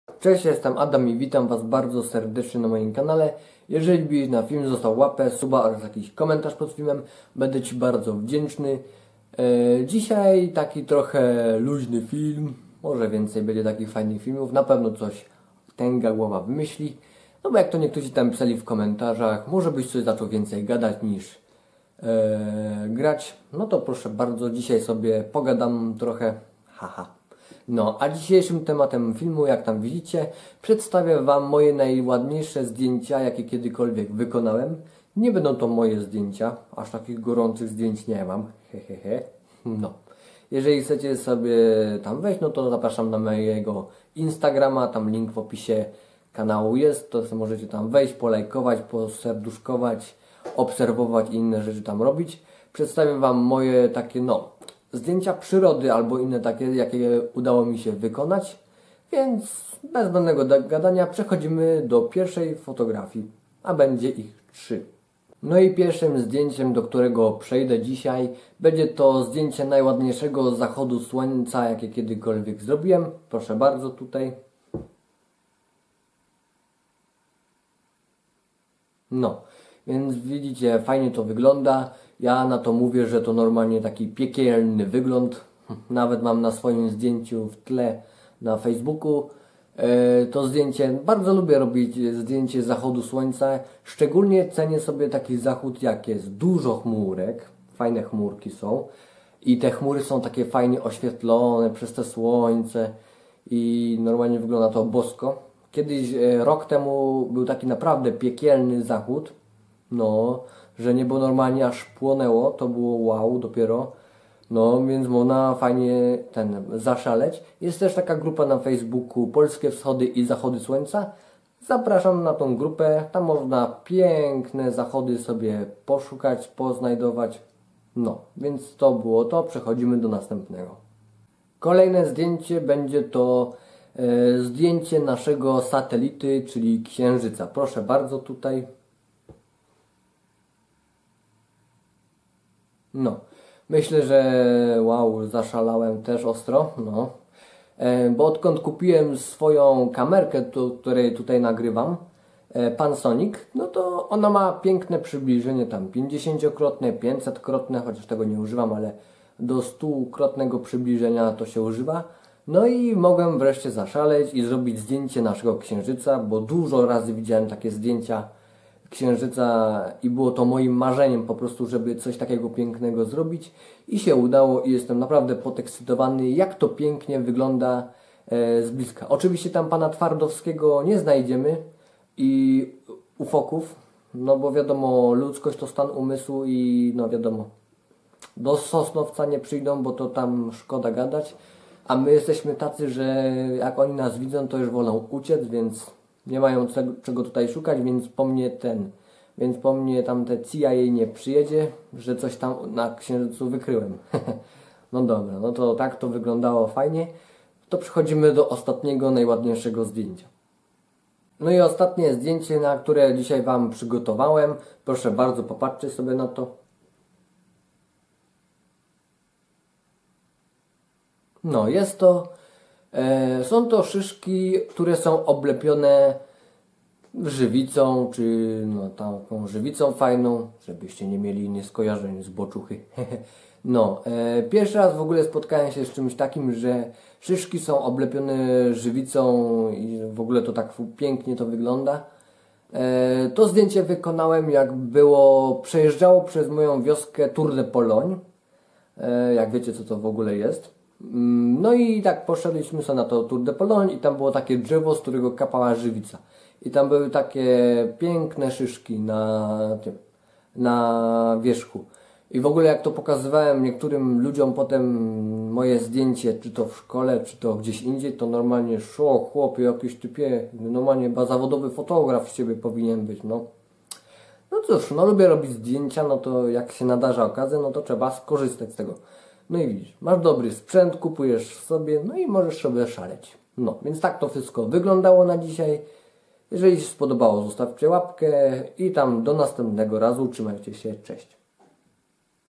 Topowa 3. Najpiękniejsze zdjęcia wykonane przeze mnie.
Siemanko! Przeczytaj opis.

Witam serdecznie na moim kanale. Dzisiaj taki luzny filmik, w którym przedstawię wam moje najpiękniejsze zdjęcia przyrody jakie wykonałem. Mam nadzieję że ci się materiał spodoba.

Sprzęt którym robiłem zdjęcia: Panasonic Full HD

Więcej zdjęć na moim insta, link w opisie kanału.

Jeśli się spodobało zostaw lajka, a jak jesteś nowy walnij dzwonka. Z góry dziękuję.